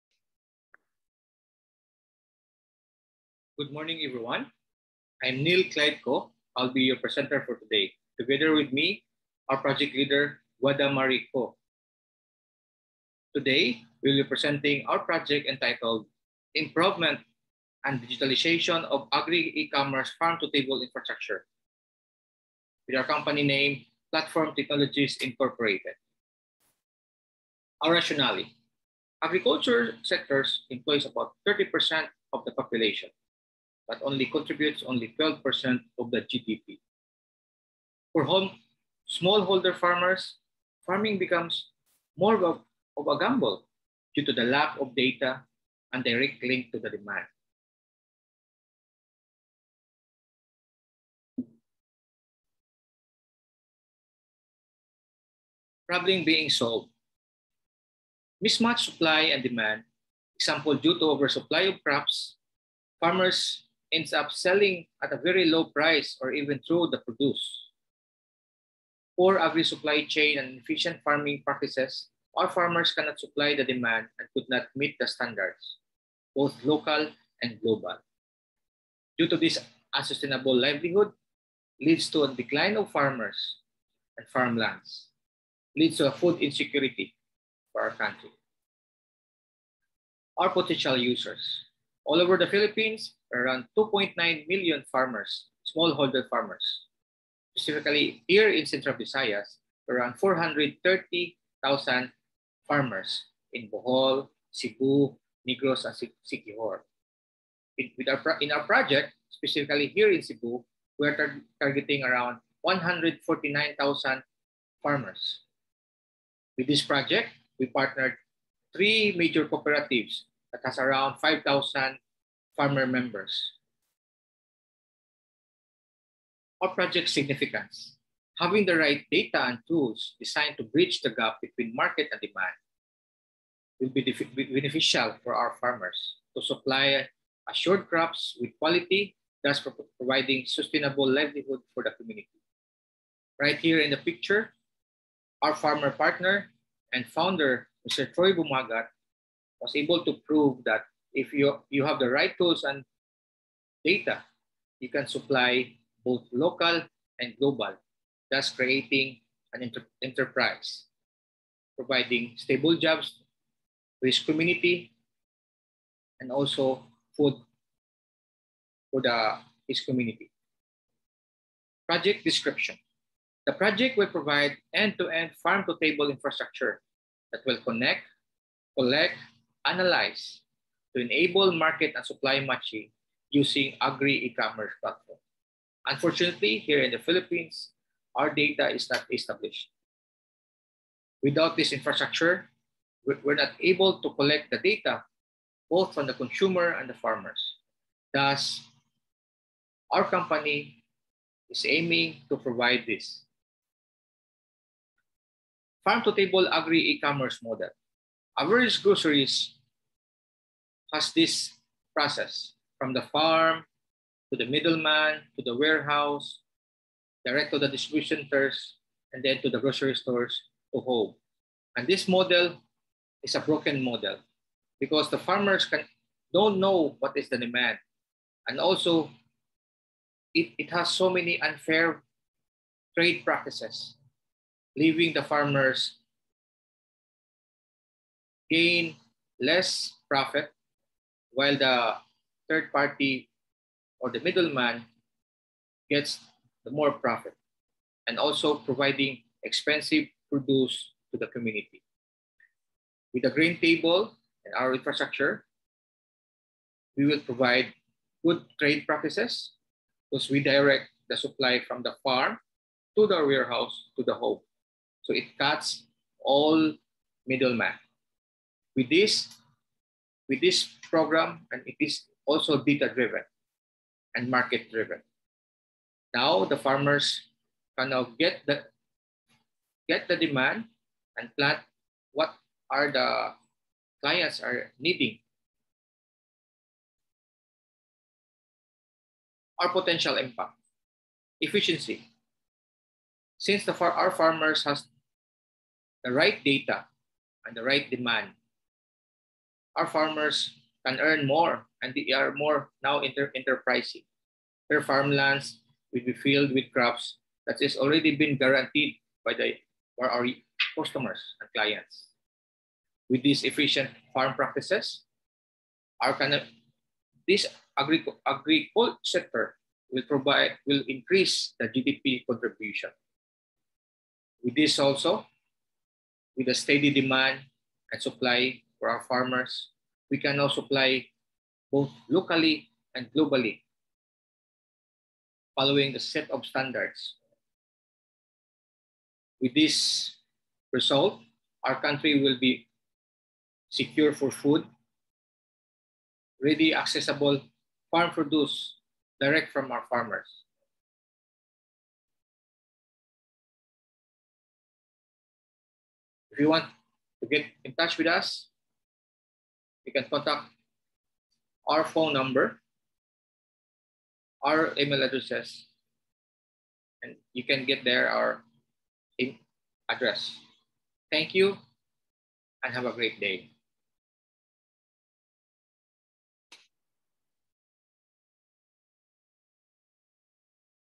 Improvement and Digitalization of Agri-Ecommerce Farm To Table Infrastructure
Having the right data and tools design to bridge the gap between market and demand will be beneficial to our farmers to supply the assured crops with quality thus providing sustainable livelihood for the community.